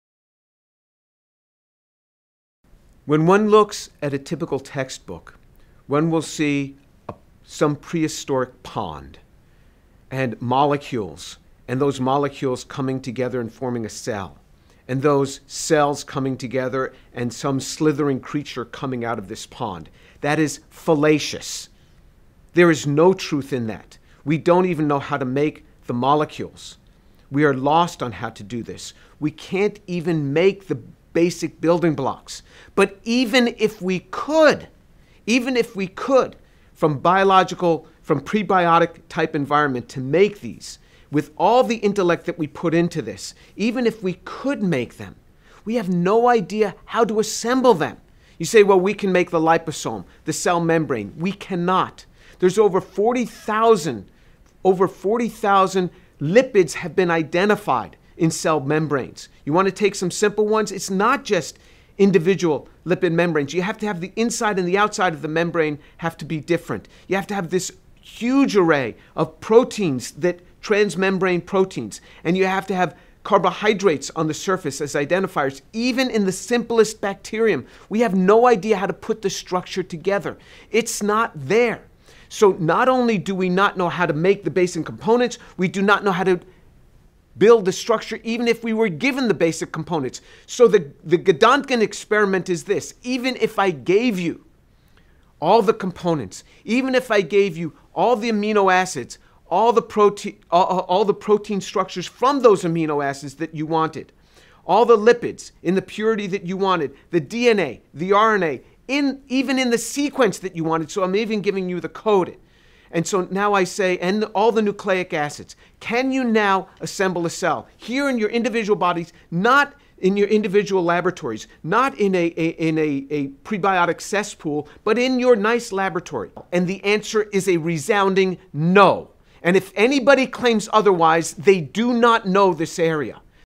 Dr James Tour tells the truth about abiogenesis
Origin of life has not been explained. Textbooks lie

-James M. Tour is an American synthetic organic chemist, specializing in nanotechnology. Tour is the T. T. and W. F. Chao Professor of Chemistry, Professor of Materials Science and NanoEngineering, and Professor of Computer Science at Rice University in Houston, Texas, United States-